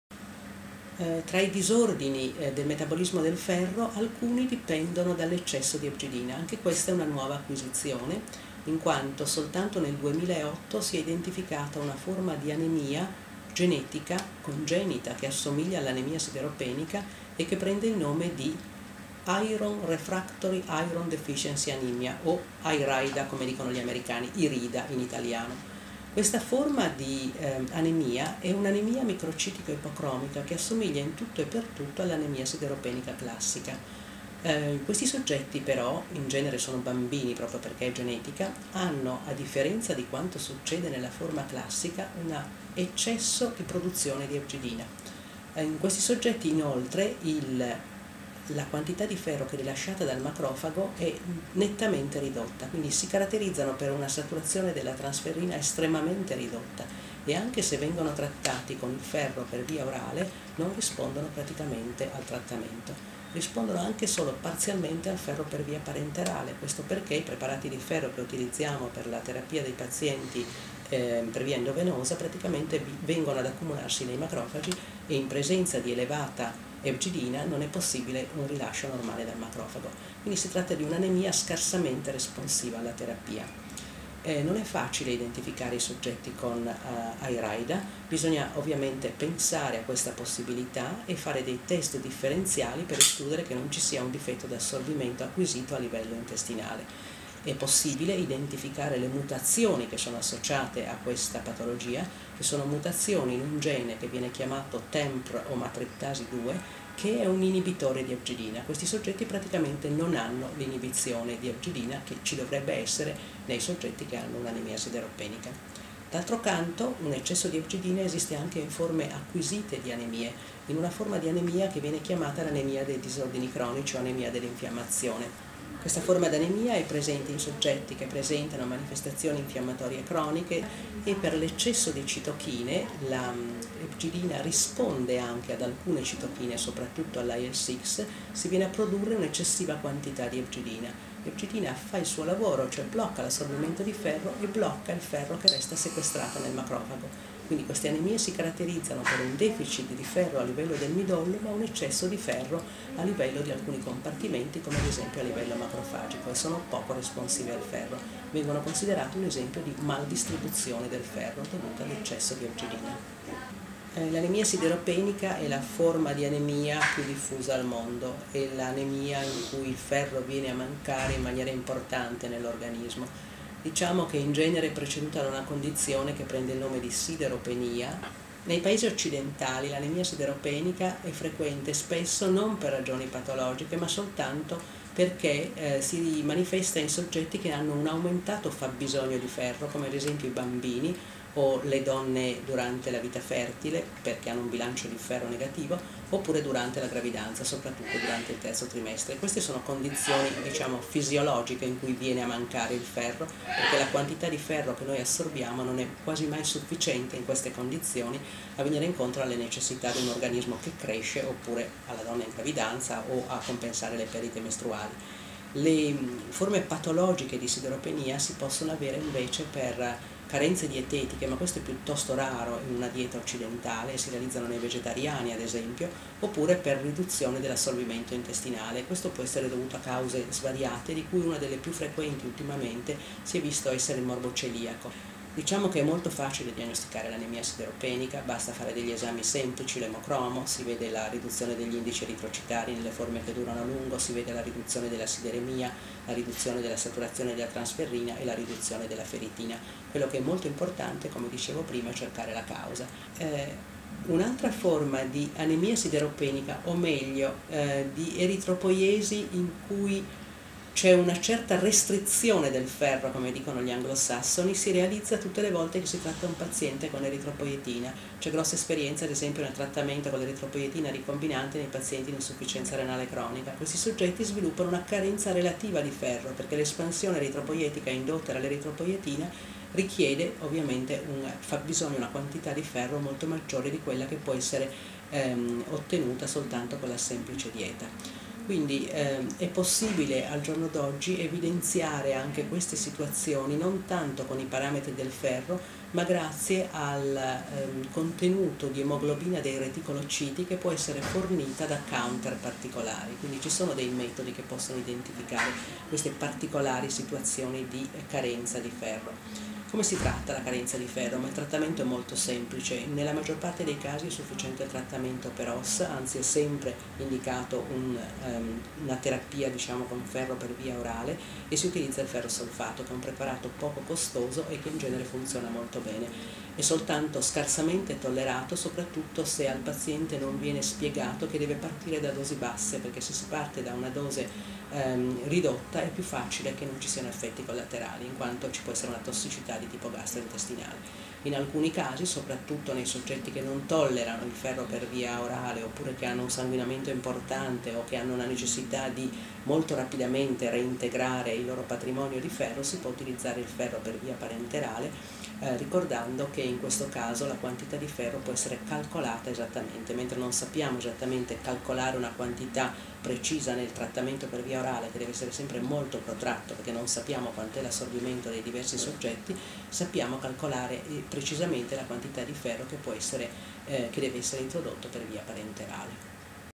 Clara Camaschella - Eccesso di epdicina, sideropenia e anemia sideropenica
L'iperproduzione di epcidina è forma di anemia microcitica estremamente rara, identificata in pazienti pediatrici affetti da glicogenosi per deficit di glucosio-6-fosfatasi. Questi pazienti possono sviluppare con il tempo adenomi epatici. Nel caso in cui gli adenomi siano ipersecernenti epcidina si instaura una anemia di grado moderato con le caratteristiche dell'anemia di tipo infiammatorio che risponde alla rimozione dell'adenoma. La sideropenia è una condizione in cui il ferro totale dell'organismo è ridotto in modo considerevole. Se questa condizione si protrae compromette l'apporto di ferro a tutti i tessuti, ma in particolare all'eritropoiesi con l'insorgenza di anemia sideropenica. L'anemia sideropenica infine è la forma di anemia più frequente in tutto il mondo. L'organizzazione mondiale della sanità stima in circa 2 miliardi i soggetti affetti nel mondo, prevalentemente nelle aree in via di sviluppo per carenze dietetiche o infestazioni intestinali